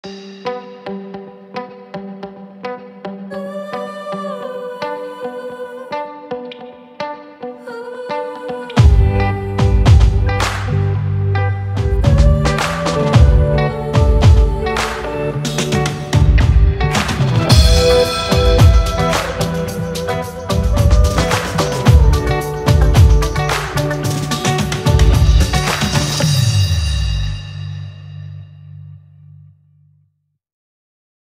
What Will I See in Wakatobi
A quick video showcasing what you can see on one of our Wakatobi snorkel safaris.